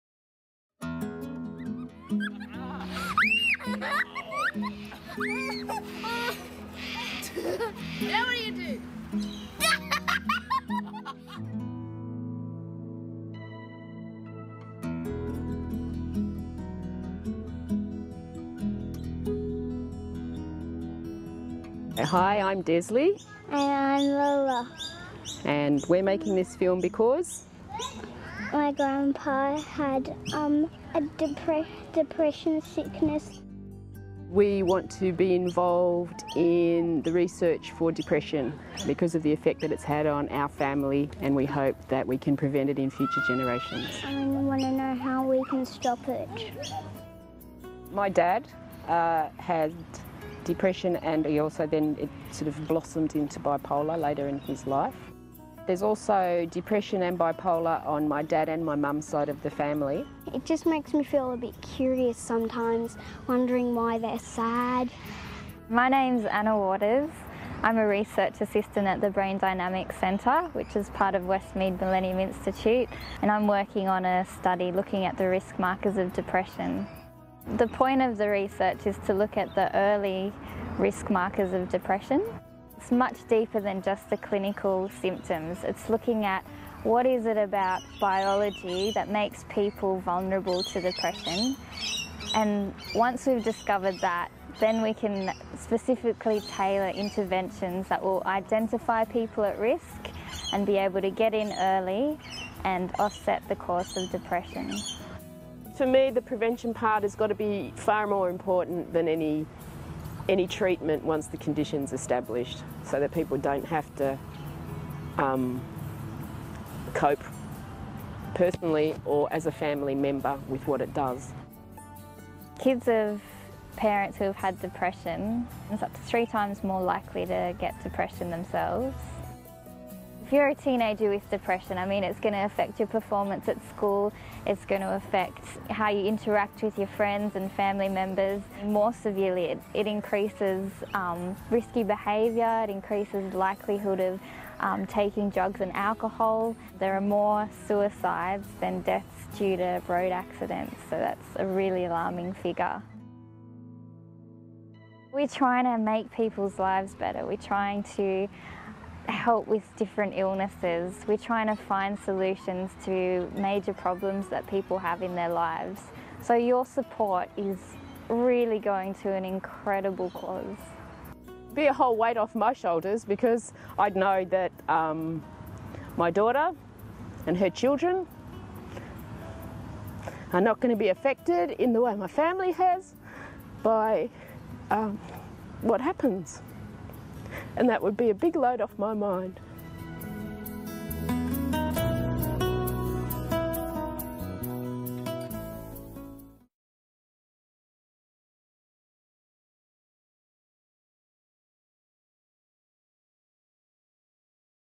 Family of Depression - A world first investigation
Did you know there are more deaths from suicide with people from depression than there are road accidents? This is alarming and the Brain Dynamics Centre in their new study is trying to identify what makes people vunerable to depression.